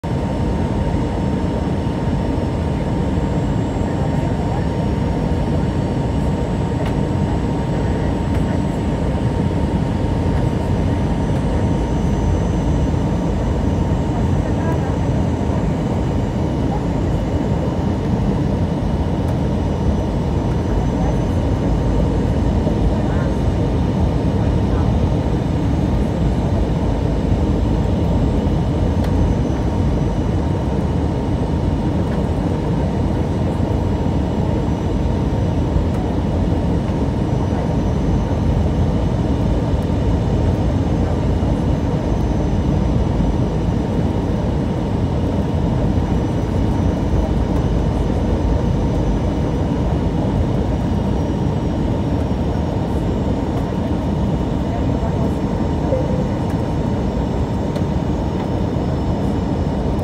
Scenic approach into Rio de Janeiro (Santos Dumont-SDU airport) | July ‘21
The approach into Rio de Janeiro via the Santos Dumont Airport (SDU) offers great views. This is the smaller, domestic airport in Rio de Janeiro appx. 20 minutes away from Copacabana.